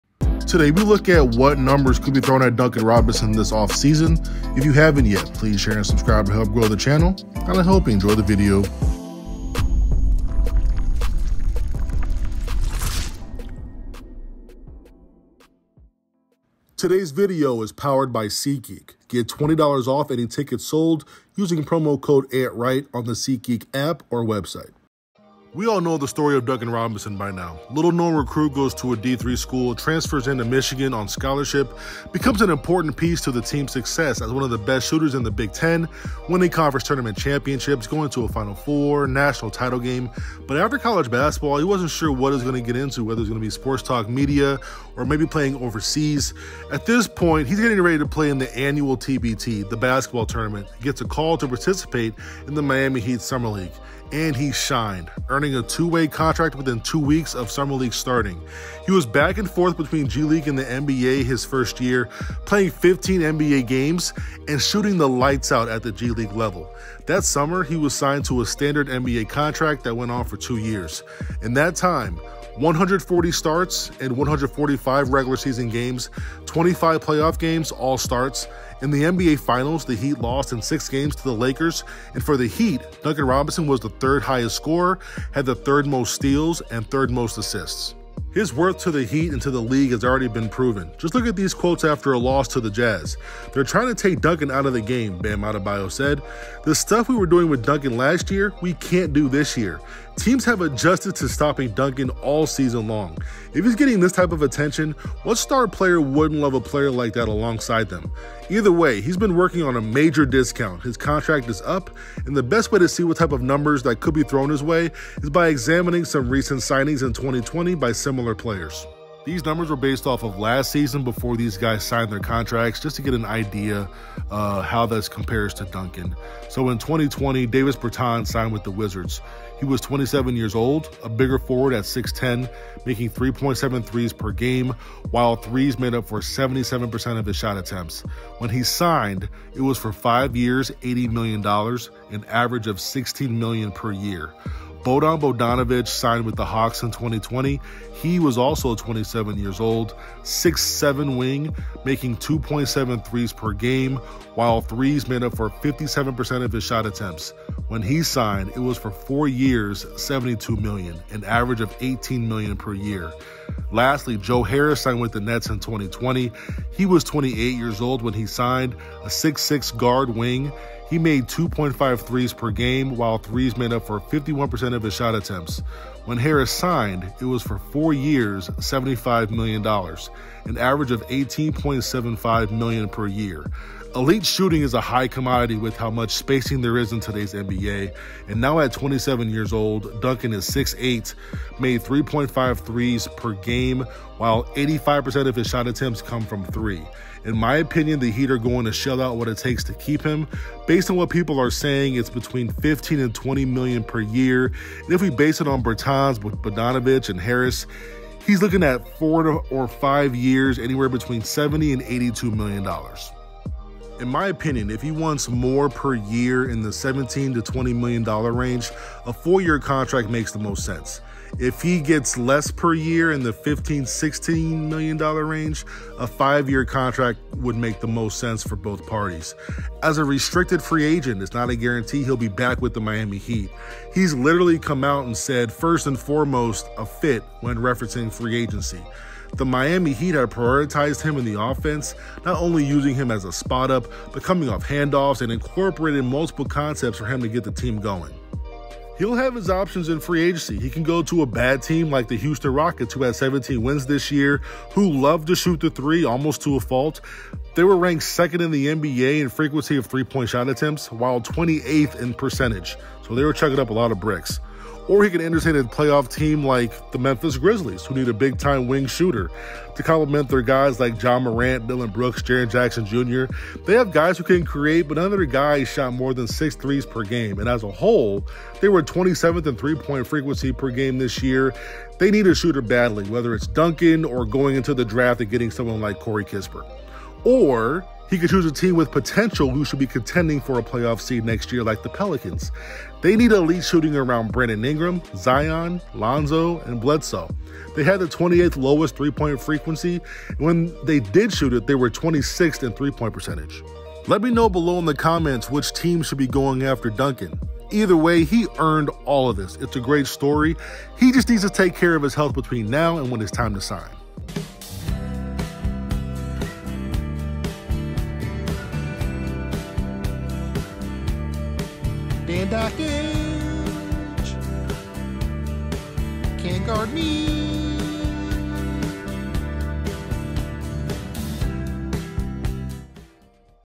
Duncan Robinson’s BIG Upcoming Contract
I look into some realistic numbers and how Duncan’s next contract could look. Film credit to Tobin, Swish Cultures, Fanatics View, Miami Heat.